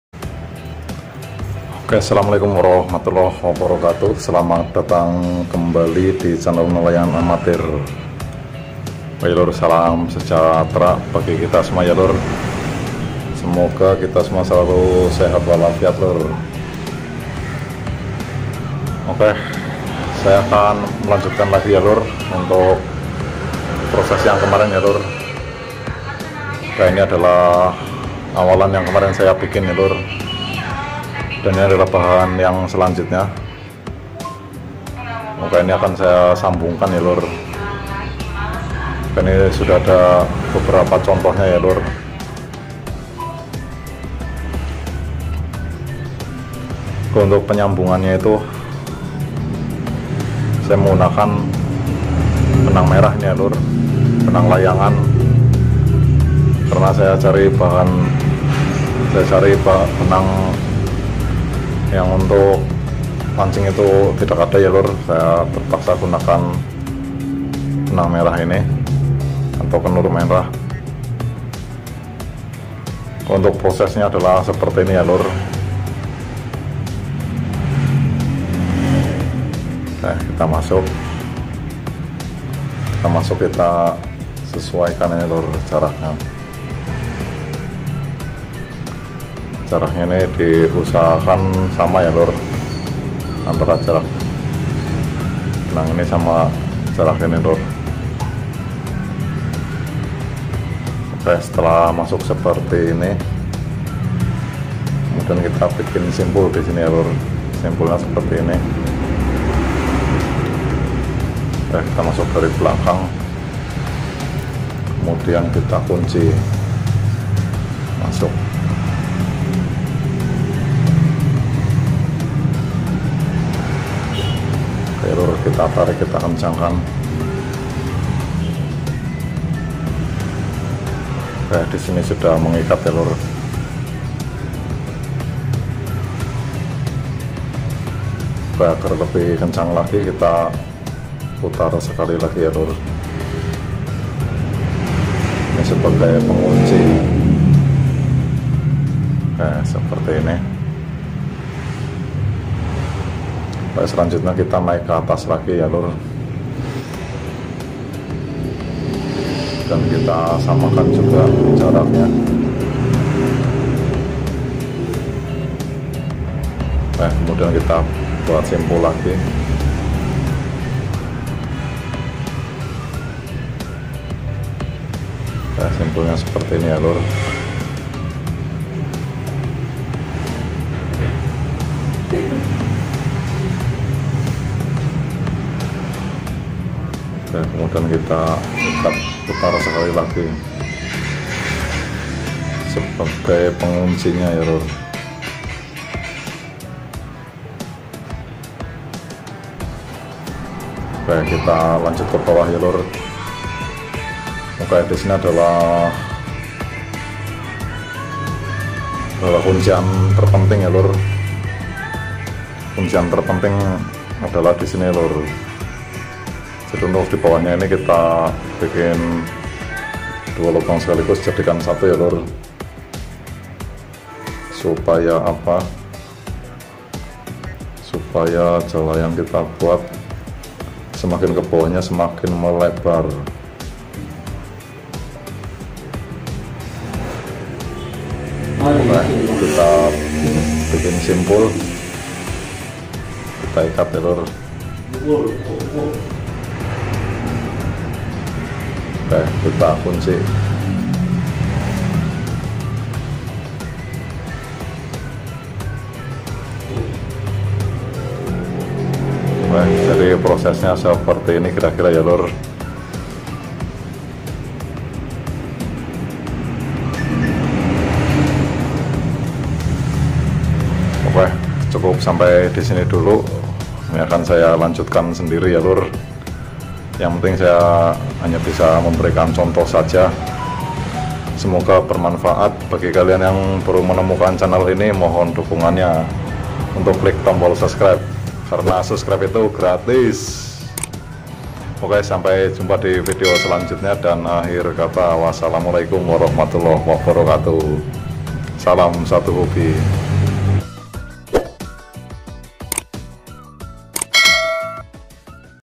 Cara menyambung dan membuat lobang tambahan"
Selamat datang kembali di channel nelayan amatir. Oke lur kali ini saya akan mencontohkan Cara membuat awalan jala ikan menggunakan bahan setengah jadi, Supaya tidak gagal faham, simak baik baik ya lur Videonya, pasti kalian bisa. Semoga bermanfaat bagi kalian, Salam satu hobi, salam seduluran.